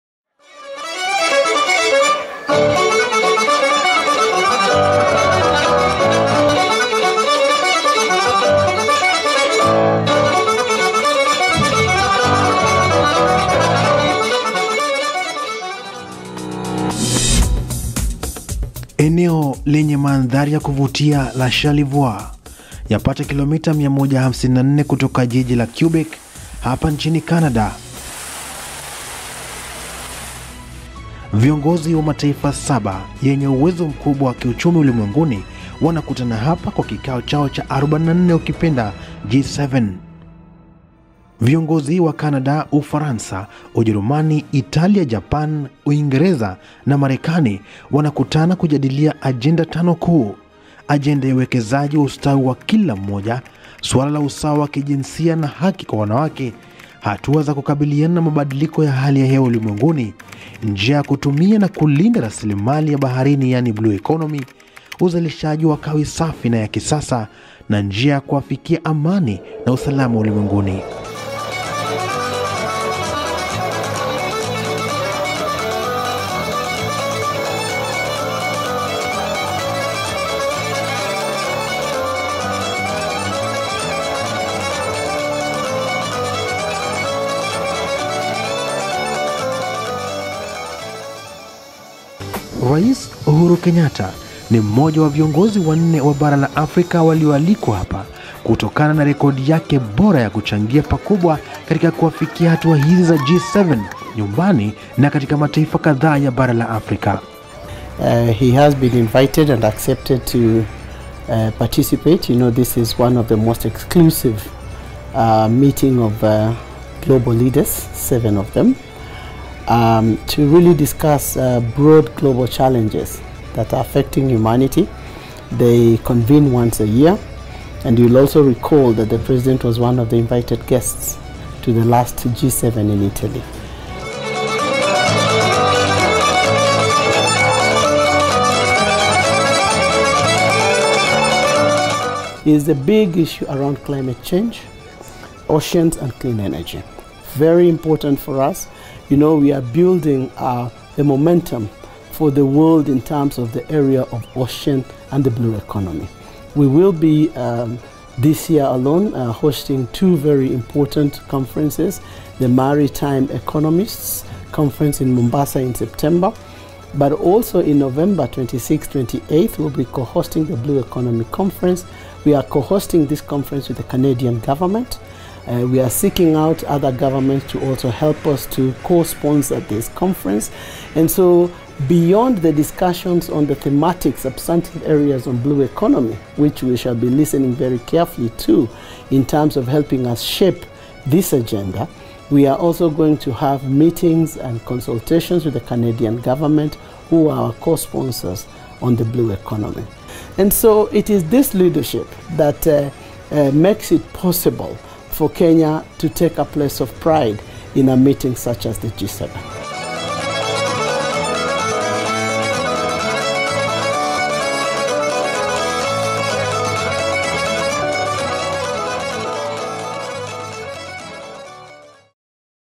G7 SUMMIT
President Uhuru Kenyatta travelled to La Malbaie in Quebec, Canada where he will attend the G7 Summit.